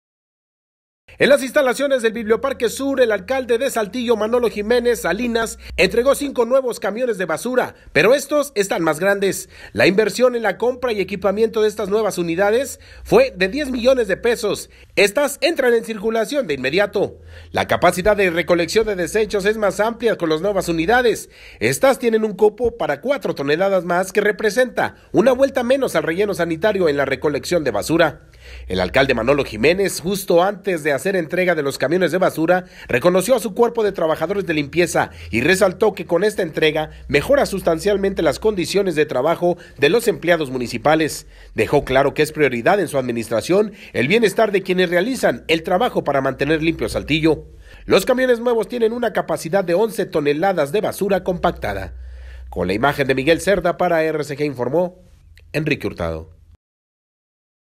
Entregan camiones de basura
En las instalaciones del biblioparque sur el alcalde entregó cinco nuevos camiones de basura, pero estos están más grandes.
La inversión en la compra y equipamiento de estas nuevas y modernas unidades fue de diez millones de pesos, estás entrena en circulación de inmediato.
La capacidad en la recolección de desechos es más amplia con las nuevas unidades, estás tiene cupo para cuatro toneladas más que representa una vuelta menos al relleno sanitario en la recolección de basura.
El alcalde Manolo Jiménez justo antes de hacer entrega de los camiones de basura, reconoció a su cuerpo de trabajadores de limpieza, resaltó que con esta entrega mejora sustancialmente las condiciones de trabajo de los empleados municipales, dejó claro que es prioridad para su administración el bienestar de quienes realizan el trabajo para
Mantener limpio a saltillo.

Los camiones nuevos tienen una capacidad de 11 toneladas de basura compactada.